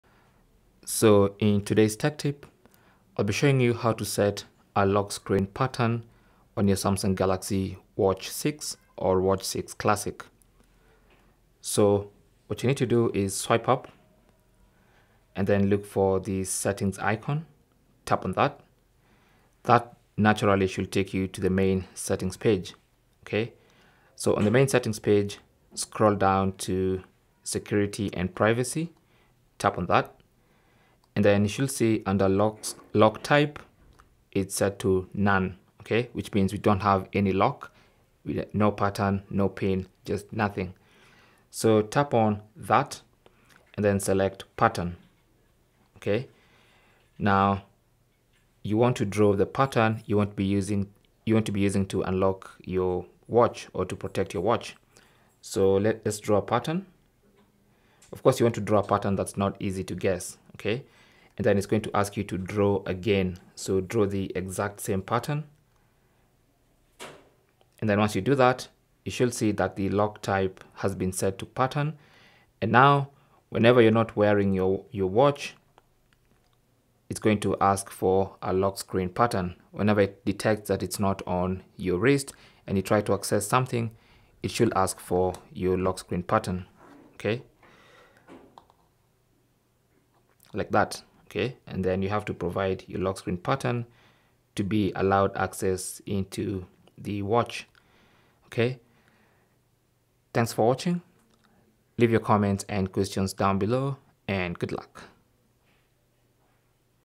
How To Set Up Lock Screen Pattern On Samsung Galaxy Watch 6 / 6 Classic
Learn how to set up a lock screen pattern on your Samsung Galaxy Watch 6 or 6 Classic! In this tutorial, we'll walk you through the process to ensure your precious data and personal information stay safe and secure.

Whether you're new to the world of smartwatches or just upgrading your device, we've got you covered.

Set Up Lock Screen Pattern On Samsung Galaxy Watch 6 / 6 Classic:

Step 1: Press the home or power button to wake up your Galaxy Watch. Swipe down and tap on the Gear Icon: This will take you to the Settings menu.

Step 2: Scroll down and select the "Security and privacy" option. This is where you can manage your device's security settings.

Step 3: Under "Lock type", you should see that "None" is the default. Likewise, any other option may be selected. Tap on this option to access the lock screen options.

Under the 'Lock Type' options, choose 'Pattern.'

Step 4: You'll be prompted to draw a pattern by connecting at least four dots. This will be your unique lock screen pattern. Ensure it's something you can easily remember but not predictable.

Step 5: After drawing your new pattern, you'll be asked to redraw it to confirm. This step ensures you didn't make any mistakes while creating your pattern.

Step 6: Lock your watch. Now, try unlocking it using your newly created lock screen pattern to ensure it works seamlessly.

Cell Phone Tripod Adapter